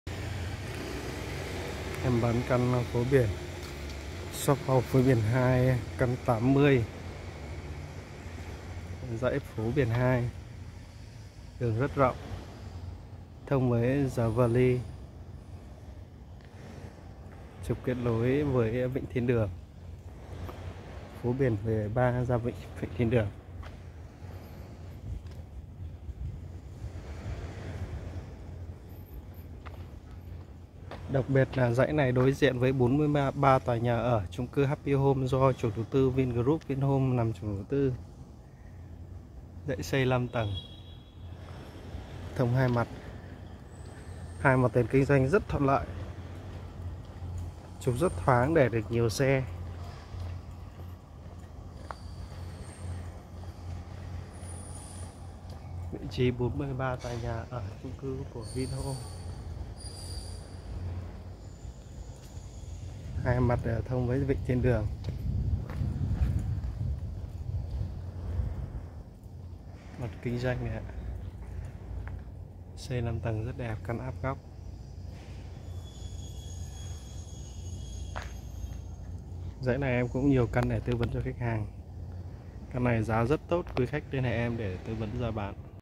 Bán shophouse Phố Biển 2 căn 80 Vinhomes Ocean Park 3 The Crown đối diện 43 tòa chung cư Happy home
Bán shophouse Phố Biển 2 căn 80 Vinhomes Ocean Park 3 The Crown Hưng Yên
Đối diện 43 toad nhà ở chung cư Happy
Dãy thông 2 mặt
Diện tích 100m2, Mt 5, hướng Đông Bắc- Tây nam
gần Vịnh Thiên Đường Paradise
Gần the Venice